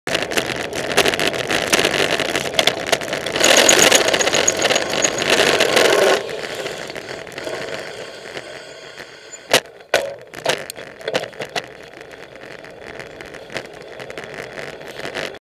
Lovely person
This lovely lady let me go, even though it was her priority. What a nice person.